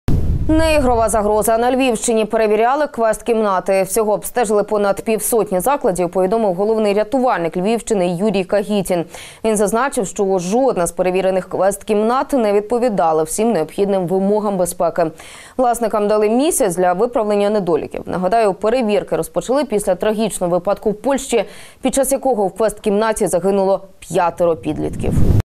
На Львівщині перевіряли квест-кімнати. ПравдаТУТ Львів
На Львівщині перевіряли квест-кімнати. Всього обстежили понад півсотні закладів, повідомив головний рятувальник Львівщини Юрій Кагітін. Він зазначив, що жодна із перевірених квест-кімнат не відповідала всім необхідним вимогам безпеки. Власникам дали місяць для виправлення недоліків. Нагадаю, перевірки розпочали після трагічного випадку в Польщі, під час якого в квест-кімнаті загинуло п’ятеро підлітків.